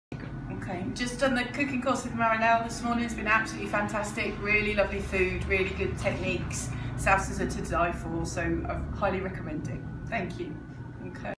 Mexican Ancestry Cooking school. Good techniques
Enjoying hands on cooking class at Marilau´s Mexican Ancestry Cooking School Classes. As a Mexican native, I inherited my Mexican cuisine knowledge from my previous generations. And my passion too! I invite you to join me in my cooking school to take out our aprons and spoons, to use the very fresh ingredients of our Mexican cuisine, and create together delicious dishes, with love, tradition and fire for a great vacation adventure. I love sharing my knowledge of ancestry home cooking in Mexico, mainly focused in hands-on learning, allowing students to choose their menus in open courses.